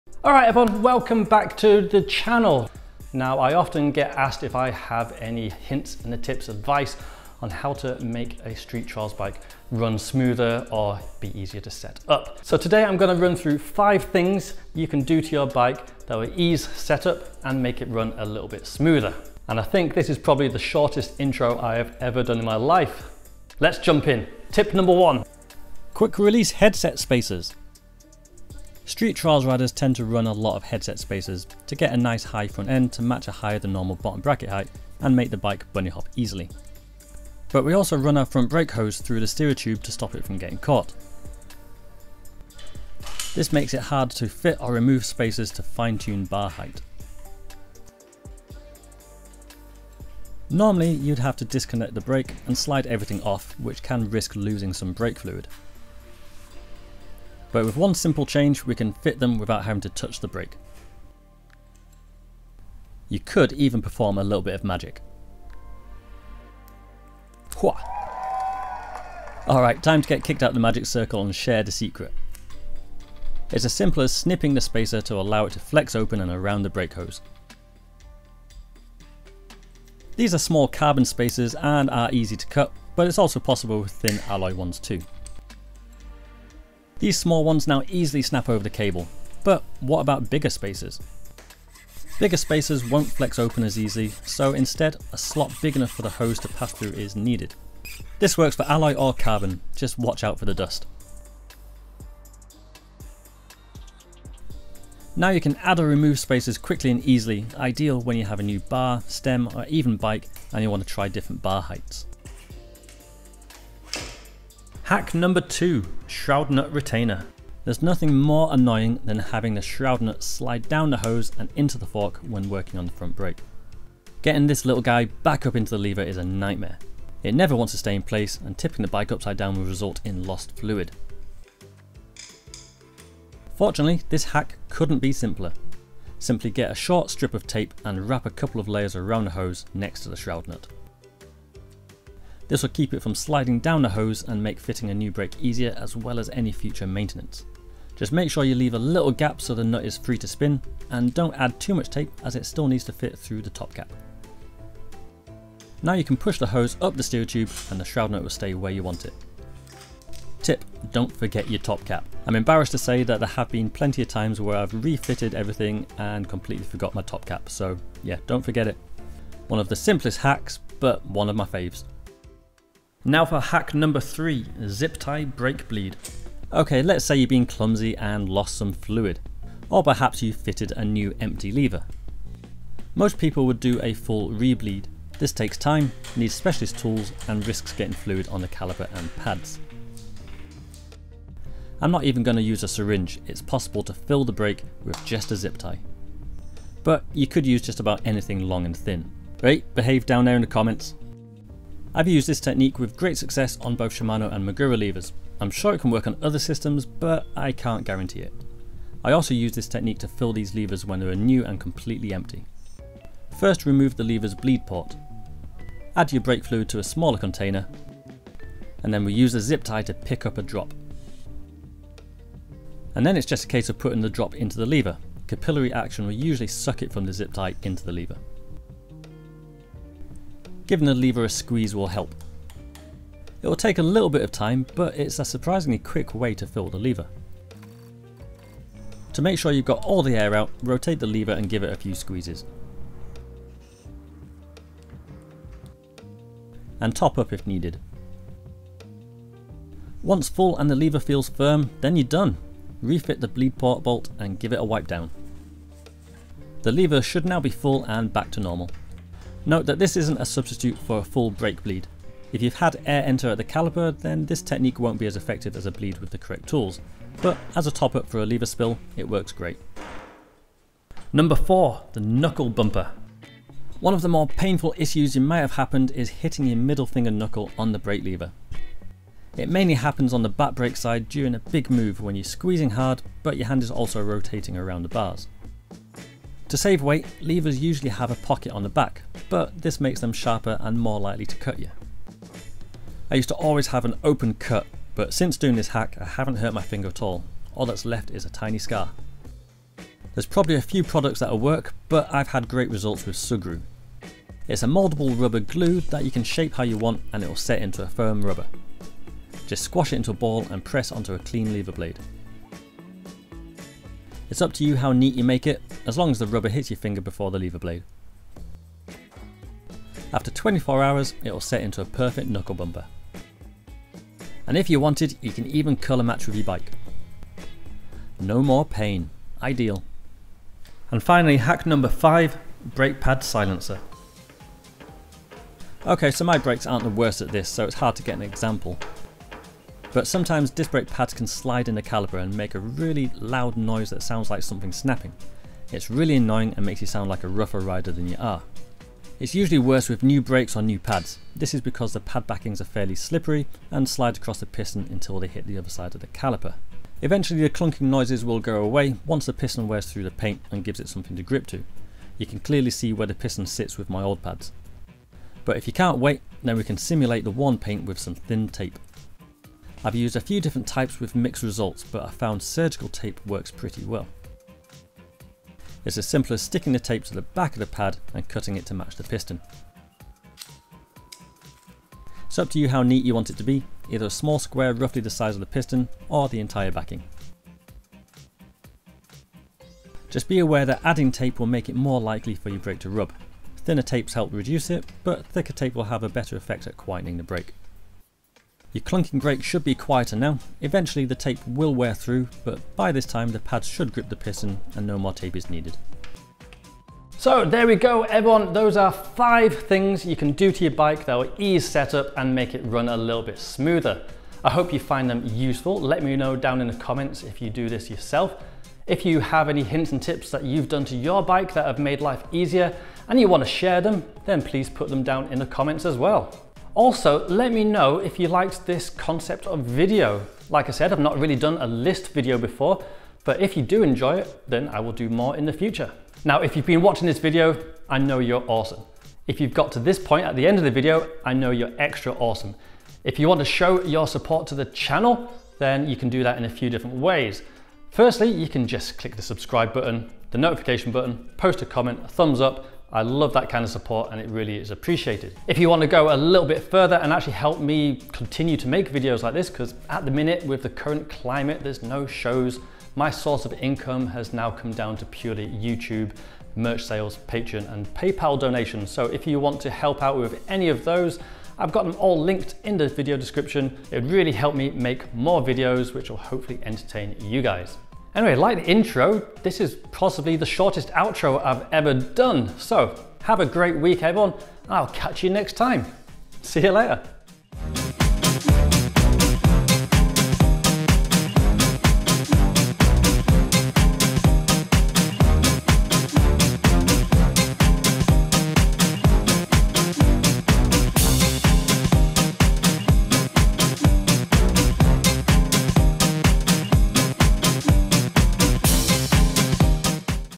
Top 5 Street Trials Bike Hacks I Always Use
I think this is the most "YouTubey" video I've ever done haha

I genuinely do get a lot of messages asking if I have any tips on bike setup, this video might not quite be what those people had in mind but these 5 things can be real time savers!

Have you used any of these tips before? Perhaps you've found some of your own hacks?